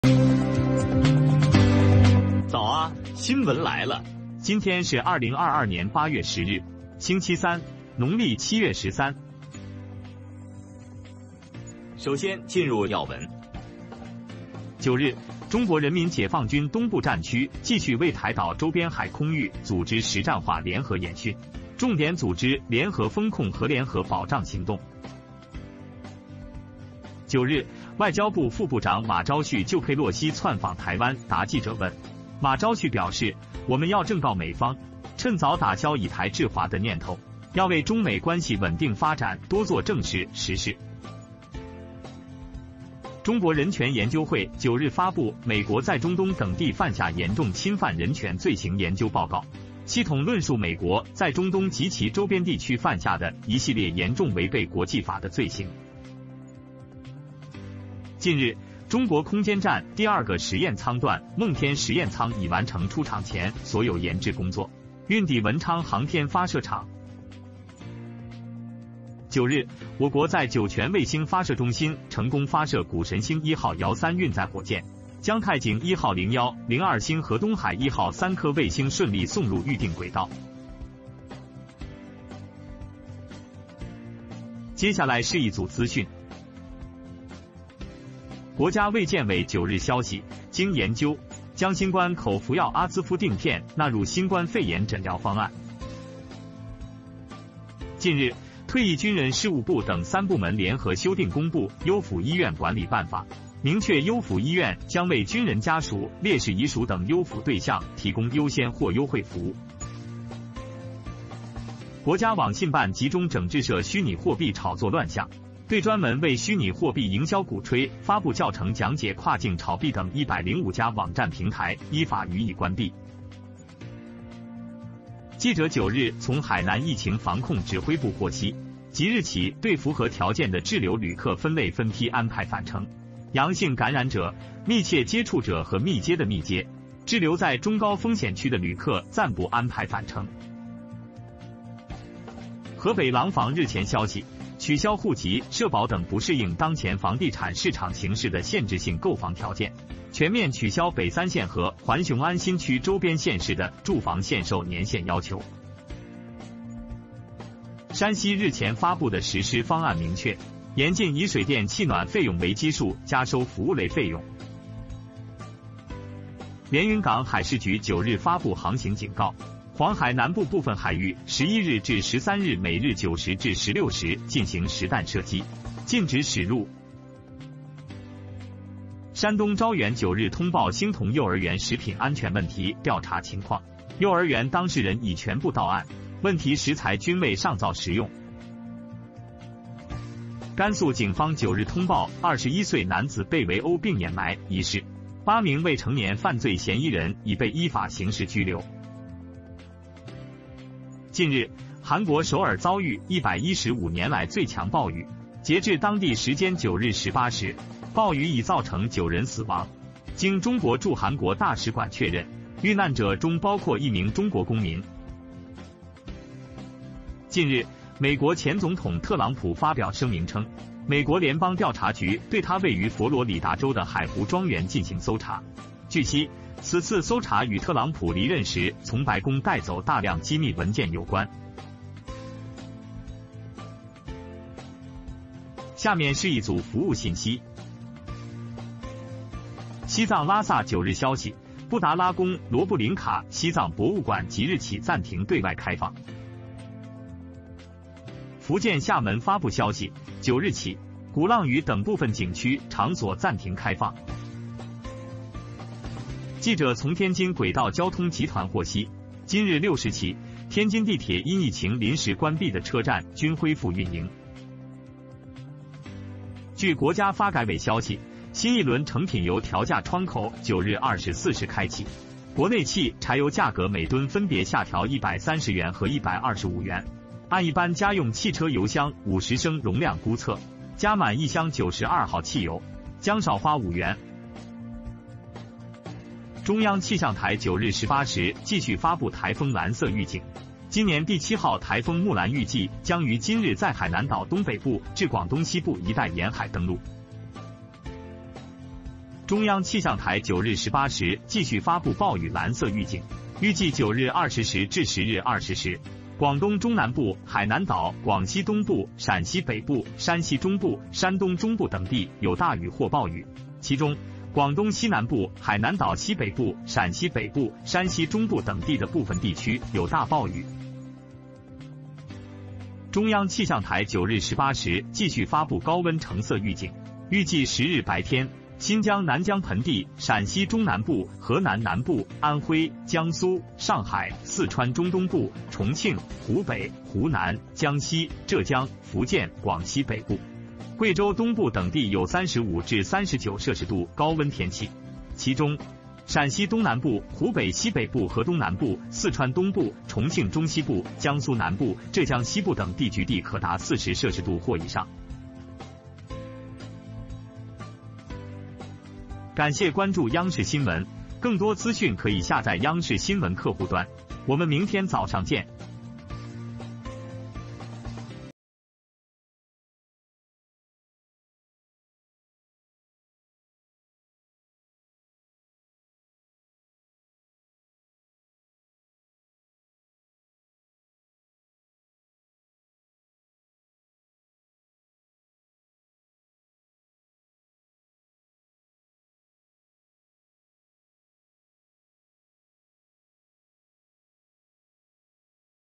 8月10日#东部战区继续位台岛周边海空域组织实战化联合演训#海南对符合条件的滞留旅客分类分批安排返程#国家卫健委新冠口服药阿兹夫定片纳入新冠肺炎诊疗方案#国内成品油价格“四连降”今年第7号台风“木兰
テキストは機械翻訳されています。　日本語⇔中国語
文本是机器翻译的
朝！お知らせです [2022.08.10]
CCTV ニュース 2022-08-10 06:49 北京に投稿

○東部戦区は引き続き台湾島周辺の海空域で実戦合同演習を実施
○海南：対象となる立ち往生した乗客の復路をまとめて手配
○国家衛生健康委員会：新型クラウン経口薬アズブジン錠が新型クラウン肺炎の診断・治療計画に含まれる
○国内精製油価格「4連安」
○今年の台風7号「ムーラン」は本日上陸の見込み

中国人民解放軍の東部戦域司令部は 9 日、台湾島周辺の海空域で共同戦闘演習を継続し、共同封じ込めと共同支援作戦の組織化に重点を置いた。

馬昭旭外務次官は9日、ペロシ大統領の台湾訪問に関する記者の質問に答えた。馬兆旭氏は、「台湾を中国の支配下に置く」という考えを一刻も早く撤廃し、中米関係の安定した発展のために、より真剣かつ現実的な行動をとるよう、米国側に警告する必要があると述べた。詳細

中国人権研究会は9日、米国が中東とその周辺地域で犯した一連の犯罪を体系的に論じた「中東などにおける米国の深刻な人権侵害」に関する調査報告書を発表した。国際法に著しく違反した。

最近、中国の宇宙ステーションの 2 番目の実験モジュールである孟田実験モジュールが、工場を出る前にすべての研究開発作業を完了し、文昌宇宙発射場に到着しました。詳細

わが国は 9 日、酒泉衛星発射センターでセレス 1 ヤオサン キャリア ロケットの打ち上げに成功し、太京 1 号 01/02 衛星と東海 1 号衛星を所定の軌道に投入することに成功しました。

画像
△Taijing-1 01/02 衛星は主に商用リモートセンシング サービスの提供に使用され、Donghai-1 衛星は主に超小型偏光カメラのマルチモード リモートセンシング検出技術の検証に使用されます。

国家保健医療委員会は9日、調査の結果、新しいクラウン経口薬アズブジンタブレットが新しいクラウン肺炎の診断と治療計画に含まれたと報告した。

最近、退役軍人省と他の 3 つの部門が共同で「特別治療病院の行政措置」を改訂して公布し、特別治療病院が兵士の家族、殉教者の生存者、およびその他の特別治療に優先または優遇サービスを提供することを明確にしました。受信者。

中国のサイバースペース管理局は、仮想通貨の投機を伴う混乱の是正に焦点を当て、仮想通貨のマーケティングを特に提唱し、国境を越えた通貨の投機を説明するチュートリアルを公開した 105 の Web サイトとプラットフォームを閉鎖しました。

記者は9日、海南防疫本部から、今後、対象となる立ち往生した乗客を分類し、バッチで戻るように手配することを知りました。陽性感染者、濃厚接触者および濃厚接触者、中リスクおよび高リスク地域で立ち往生している乗客は、当面の間、帰国の手配を行いません。詳細

数日前、河北省廊坊は、現在の不動産市場の状況に適していない戸籍や社会保障（個人税）などの制限的な住宅購入条件が取り消され、「三北省」の住宅販売制限が解除されたと報告した。県」と雄安新区周辺の県（市）は完全に解除された。

数日前に山西省が発表した実施計画では、水、電気、暖房費に基づいてサービス料金を請求することは固く禁じられていることが明らかになりました。

連雲港海上安全局は9日、航行警報を発した。11日から13日まで、黄海南部で毎日9時から16時まで実弾が発射される。 、立ち入り禁止です。

山東省招遠市は9日、興通幼稚園の食品安全問題の調査について、幼稚園関係者全員が現場に到着し、問題の食材をストーブの上で食べたことはないと報じた。詳細

甘粛省警察は9日、「21歳の男が殴打されて埋葬された」と報じ、少年容疑者8人が法律に従って刑事拘束された。

最近、韓国のソウルは 115 年ぶりの暴風雨に見舞われ、現地時間 9 日 18 時現在、集中豪雨により 9 人の死者が出ました。韓国の中国大使館は、犠牲者の中に中国人が含まれていることを確認した。 』

最近、トランプ前大統領は、FBIがフロリダにあるマー・ア・ラーゴの邸宅を捜索したとの声明を発表した。この捜索は、トランプ氏が退任時にホワイトハウスから持ち出した大量の機密文書に関連していると報告されています。

チベット・ラサは9日、ポタラ宮、ノルブリンカ、チベット博物館が今後臨時休館すると報じた。

福建省アモイは9日から、コロンス島などの一部の景勝地や場所を一時的に閉鎖すると発表した。

記者は、天津鉄道交通グループから、今日の6:00から、流行のために天津地下鉄が一時的に閉鎖したすべての駅が営業を再開したことを知りました。

国家発展改革委員会によると、9日24時から精製油の価格調整窓口が新たに開かれた。国内のガソリンとディーゼルの価格は、トン当たりそれぞれ130元と125元引き下げられた。一般的な家庭用自動車の50L燃料タンクの推定容量によると、92号ガソリンをタンクに入れると5元安くなります。

中央気象台は9日18時、青色の台風警報を発令し続けました。今年の台風7号「ムーラン」は今日、海南島北東部から広東省西部にかけての海岸に上陸する見込みです。

中央気象台は9日18時、青色暴風雨警報を発令し続け、広東中南部、海南島、広西東部、陝西北部、山西中部などで大雨または大雨になる見込みです。山東省中部で9日20時から10日20時まで、広西南部と西部、海南島北西部、陝西北部、山西中部の一部で激しい暴風雨があります。

中央気象台は 9 日 18 時にオレンジ色の高温警報を発令し続け、10 日日中は新疆盆地南部、陝西省中南部、河南省南部、安徽省、江蘇省、上海、四川省中部および東部、重慶、湖北省、湖南省、江西省、浙江省、福建省、広西北部、貴州省東部およびその他の場所では、35〜39℃の高温気候があり、その中で、陝西省南東部、湖北省北西部および湖北省南東部、四川省東部、重慶の中西部、江蘇南部、浙江西部などの地域では、局所的に 40 °C または 40 °C に達することがあります。

▌ この記事のソース: CCTV ニュース WeChat 公式アカウント (ID: cctvnewscenter)